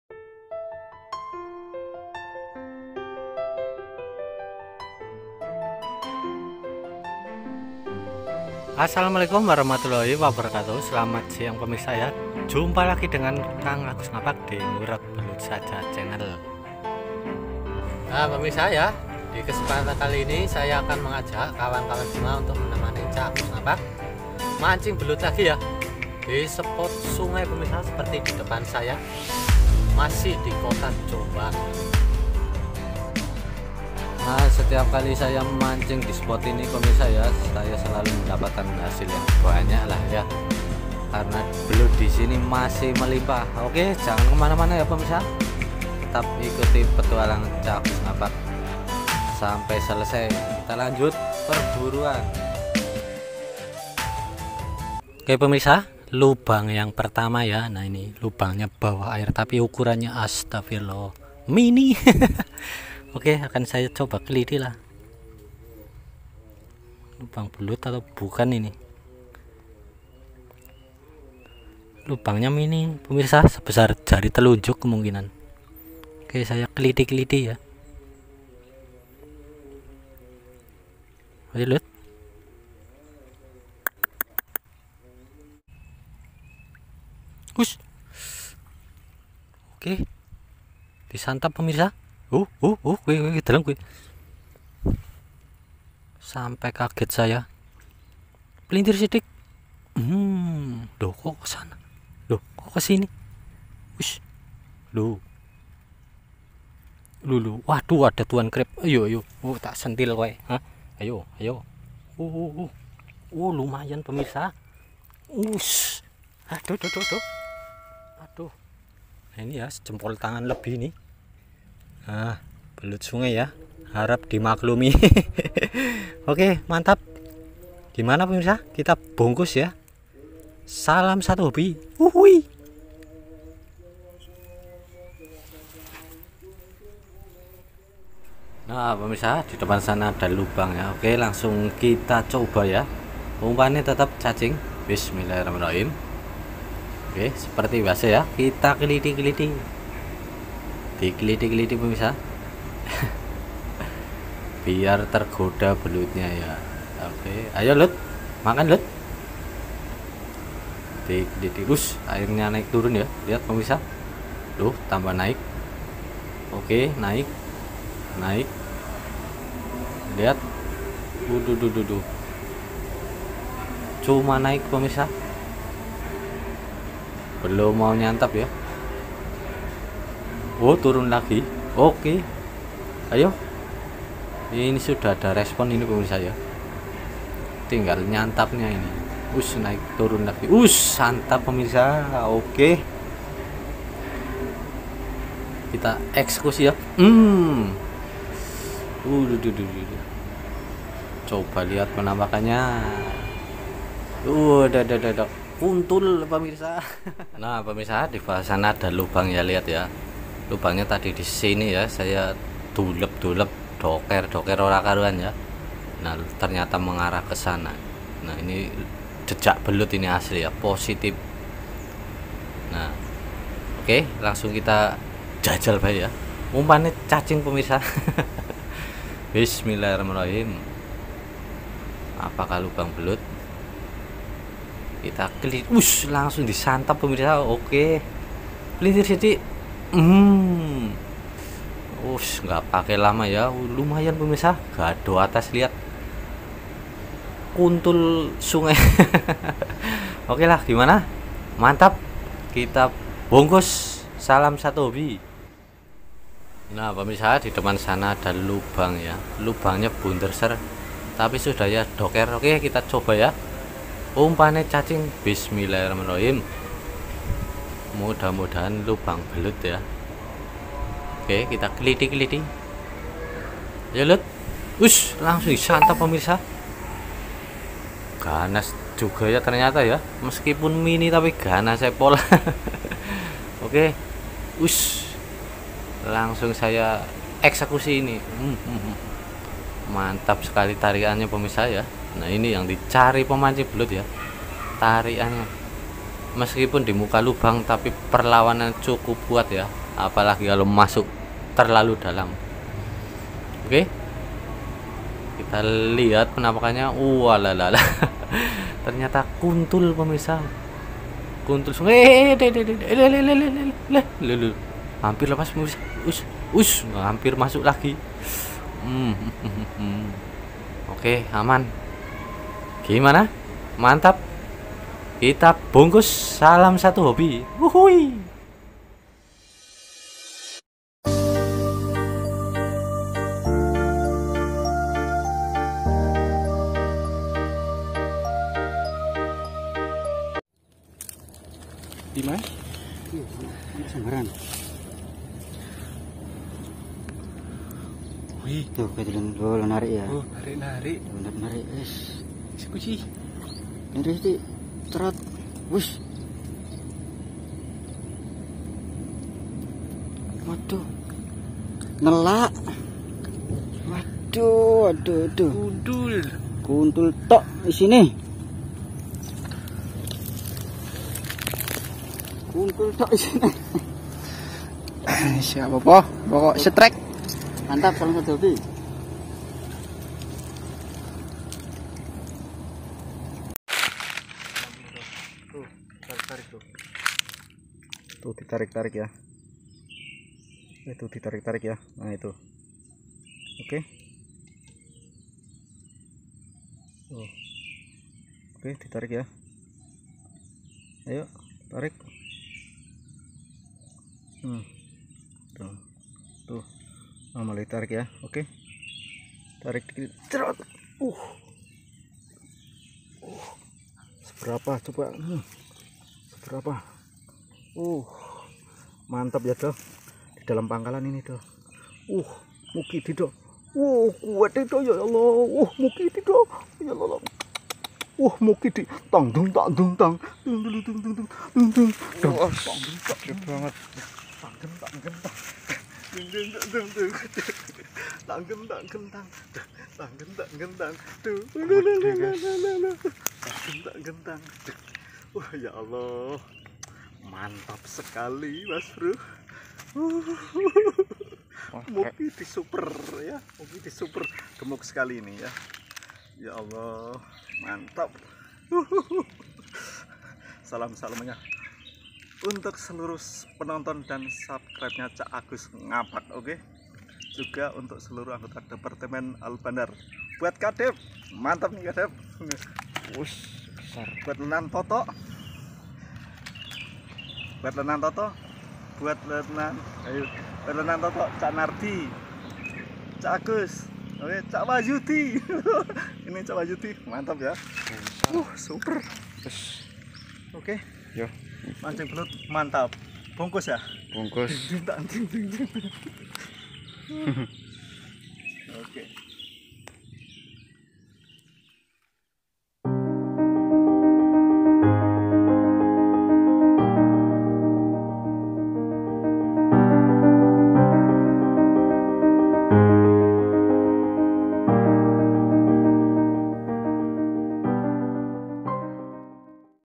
SANGAT MUDAH !!! TIAP LUBANG PASTI ADA BELUTNYA
Nah petualangan mancing belut cak Agus Ngapak kali ini di spot sungai pemirsa...